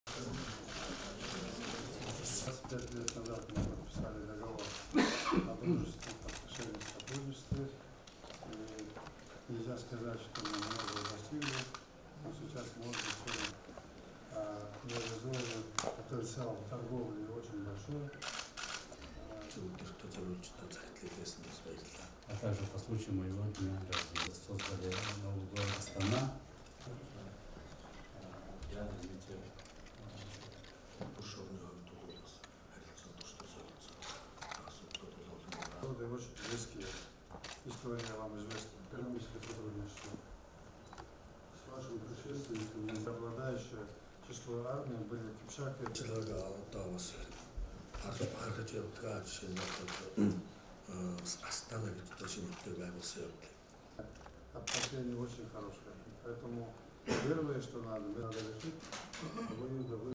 Монгол Улсын Ерөнхийлөгч Х.Баттулга БНКазУ-ын Ерөнхийлөгч Н.А.Назарбаевтай уулзав
Монгол Улсын Ерөнхийлөгч Х.Баттулга Бүгд Найрамдах Казахстан улсын Ерөнхийлөгч Н.А.Назарбаевтай уулзав